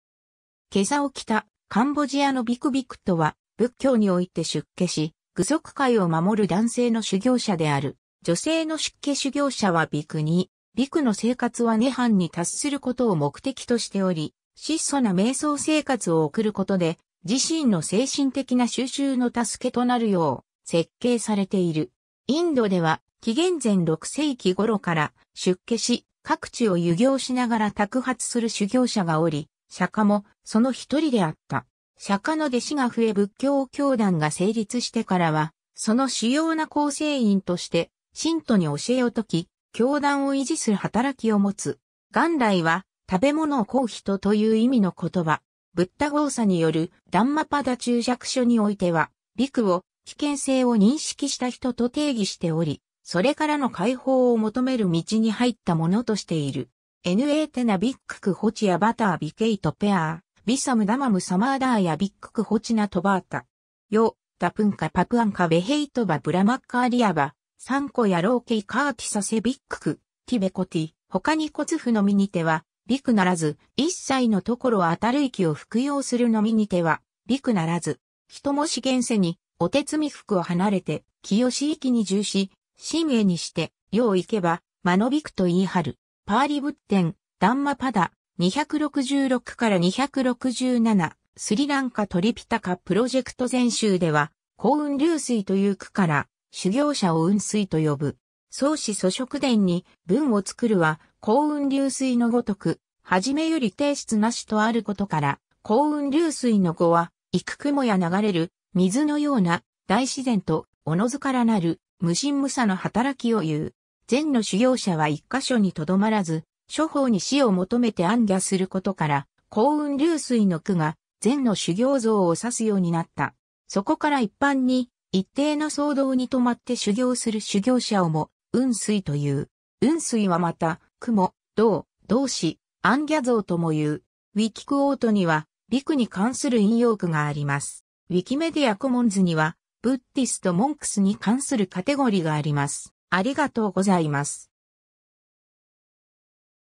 比丘
袈裟を着たカンボジアの比丘 比丘（びく、巴: Bhikkhu、梵: Bhikṣu）とは、仏教において出家し、具足戒を守る男性の修行者である。
女性の出家修行者は比丘尼（びくに, Bhikkhunī）。
 比丘の生活は涅槃に達することを目的としており、質素な瞑想生活を送ることで、自身の精神的な修習（バーヴァナー）の助けとなるよう設計されている。
 インドでは紀元前六世紀ごろから、出家し、各地を遊行しながら托鉢する修行者がおり、釈迦もその一人であった。
釈迦の弟子が増え仏教教団（サンガ）が成立してからは、その主要な構成員として、信徒に教えを説き、教団を維持する働きをもつ。
 元来は「食べ物を乞う人」という意味の言葉。
 ブッダゴーサによるダンマパダ注釈書においては、比丘を「（サンサーラや転生の輪の）危険性を認識した人」 (Pāli: Bhayaṃ ikkhatīti: bhikkhu)と定義しており、それからの解放を求める道に入った者としている。
 Na tena bhikkhū hoti yāvatā bhikkhate pare; Vissaṃ dhammaṃ samādāya bhikkhu hoti na tāvatā. Yo'dha puññca pāpañca bāhetvā brahmacariyavā; Saṅkhāya loke carati sa ce bhikkhū'ti vuccati. 他に乞ふのみにては比丘ならず、一切の所應行を服膺するのみにては比丘ならず。
 人若し現世に於て罪福を離れて淨行に住し、愼重にして世を行けば眞の比丘と謂はる。
 —パーリ仏典, ダンマパダ,266-267, Sri Lanka Tripitaka Project 禅宗では、行雲流水という句から、修行者を雲水（うんすい）と呼ぶ。
宋史蘇軾伝に「文を作るは行雲流水の如く、初めより定質なし」とあることから、行雲流水の語は、行く雲や流れる水のような、大自然とおのずからなる無心無作のはたらきをいう。
禅の修行者は一か所にとどまらず諸方に師を求めて行脚することから、行雲流水の句が禅の修行僧をさすようになった。
そこから一般に、一定の僧堂に留まって修行する修行者をも雲水という。
雲水はまた、雲衲（うんのう：なお、衲は、雲水の着る破れ衣のこと）、衲子（のっす）、行脚僧ともいう。
   ウィキクォートには、比丘に関する引用句があります。
   ウィキメディア・コモンズには、Buddhist monksに関するカテゴリがあります。